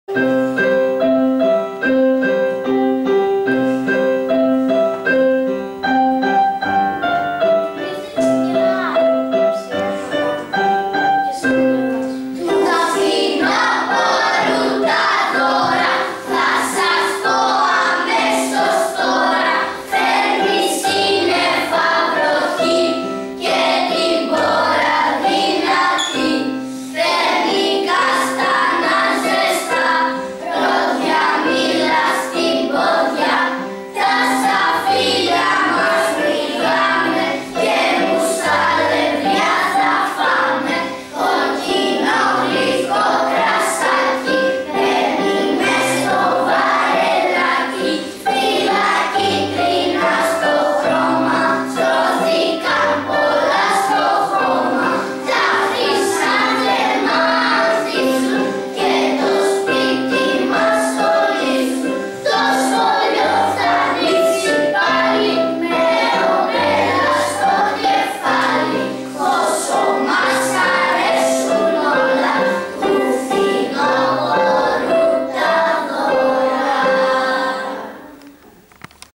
Του Φθινόπωρου τα δώρα
Ένα ποίημα για το φθινόπωρο από τους μαθητές του Β2, ντυμένο μουσικά από τον δάσκαλο της μουσικής Κασκάτη Κωνσταντίνο.